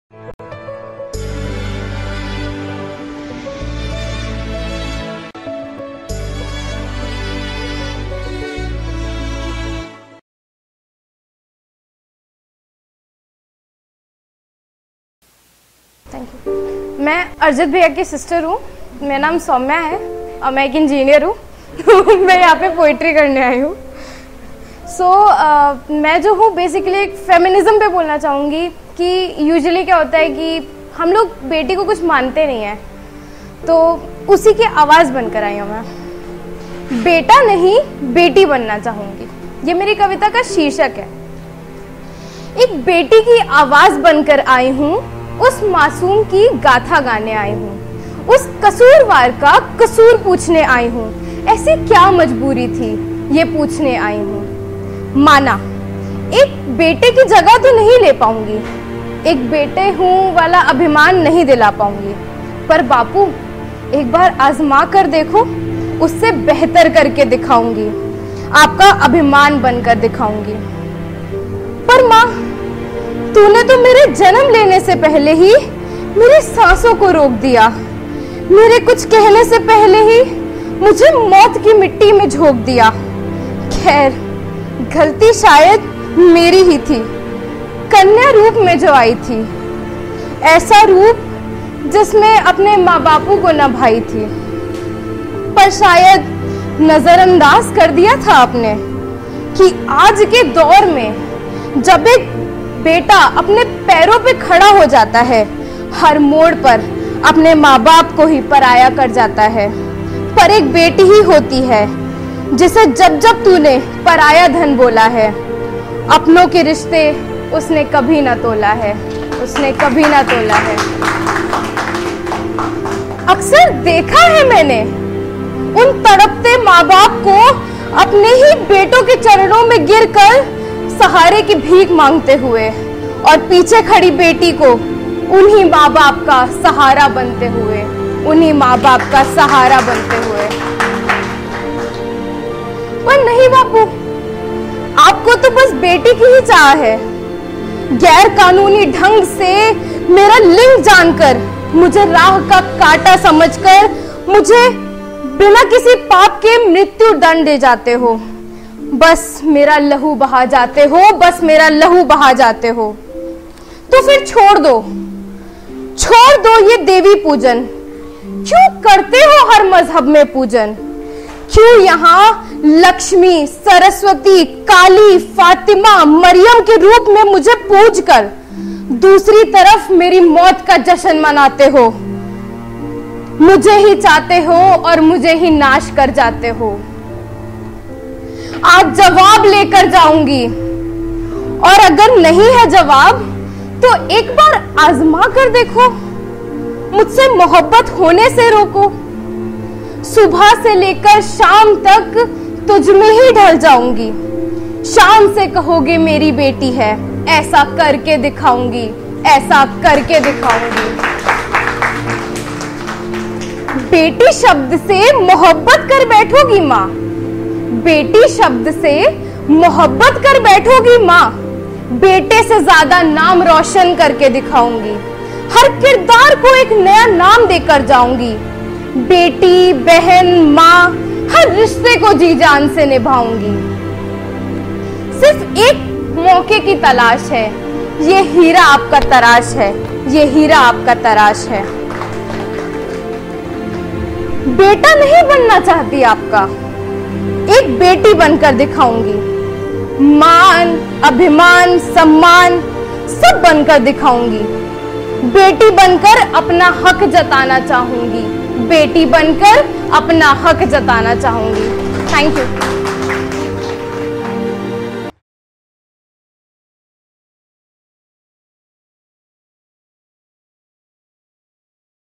THE UNHEARD || BETA NAHI BETI BANNA CHAHUNGI || SAUMYA || LUCKNOW 3.0
Let your emotions speak Share with us what you have different from others and let it not remain UNHEARD.

Powered By : Brewbakes Cafe, Aliganj Lucknow